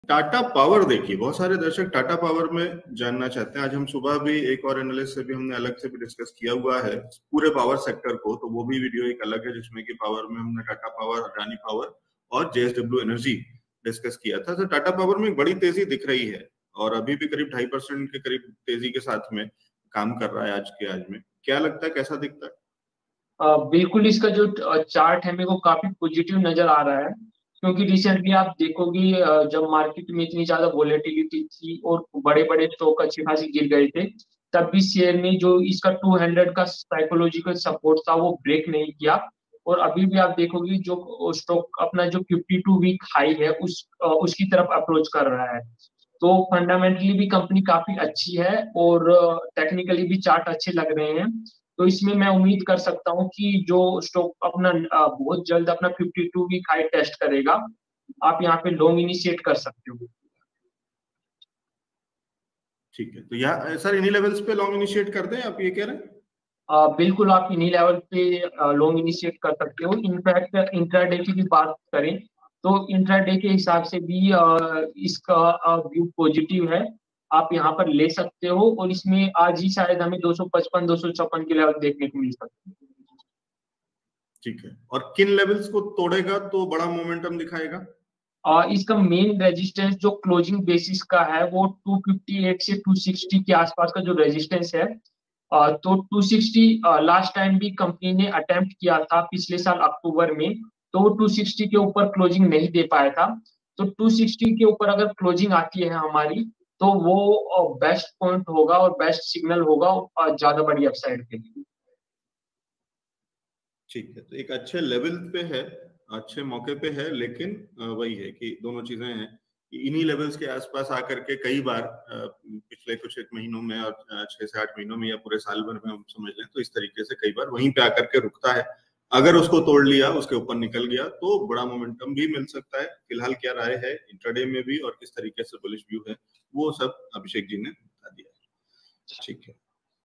Tata Power के शेयर में क्या करें? Expert Opinion by Abhishek Ninaniya
जानिए हमारे stock market expert Abhishek Ninaniya की राय |

Analysis of: TATA Power